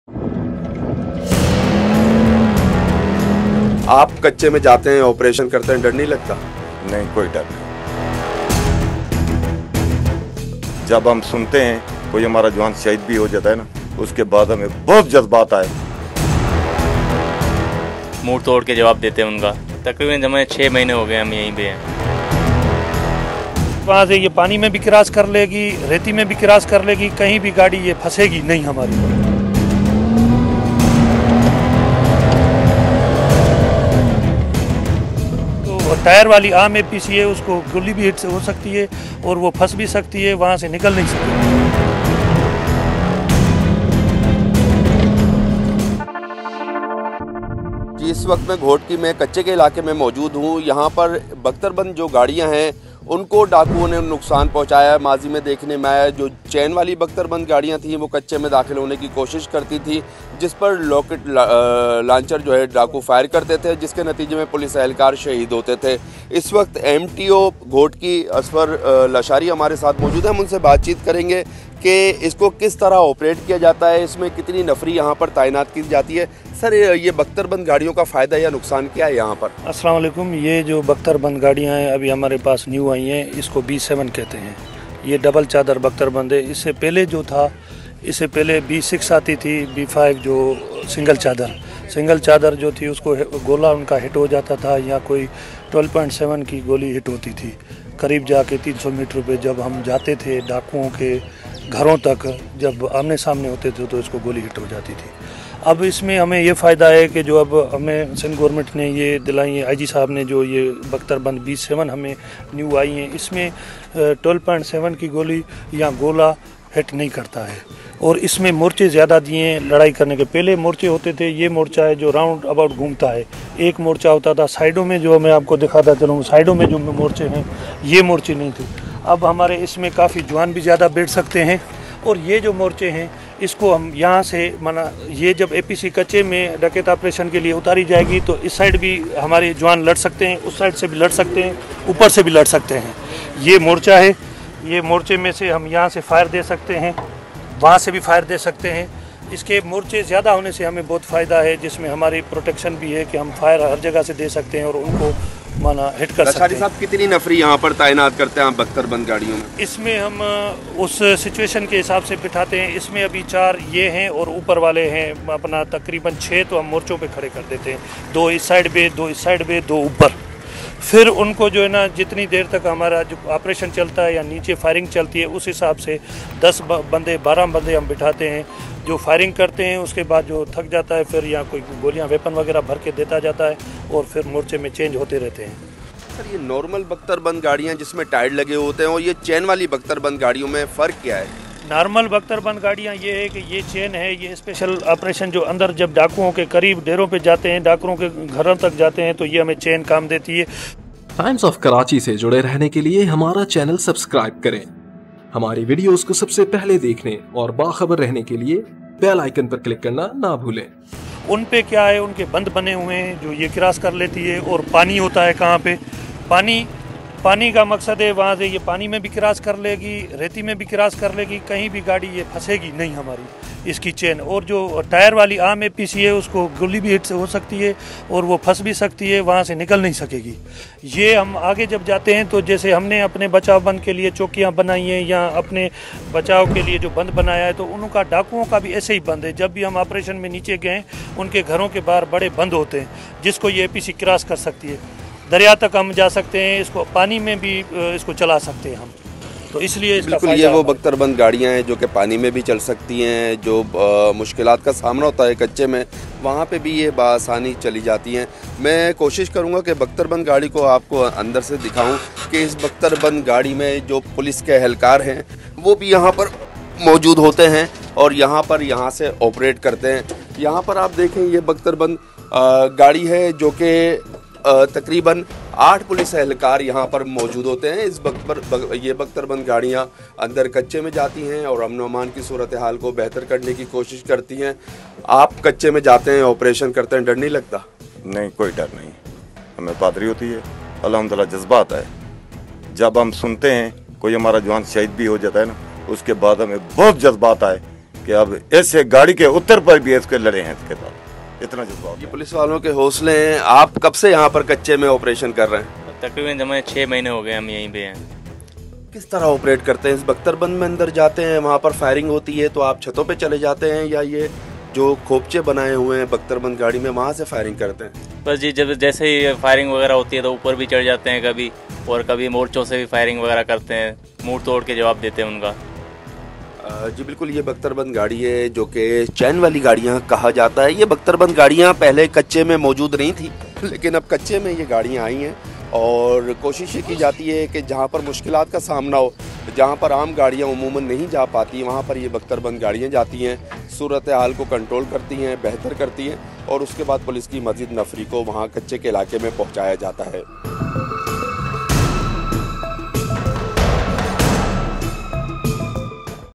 Kachey k ilaqey main Sindh police ki jadeed Baktarband main karwai
Here’s how Sindh police using new Baktarband vehicles in remote areas of Ghotki during operation. Senior crime reporter Taha Obaidi reports from the location.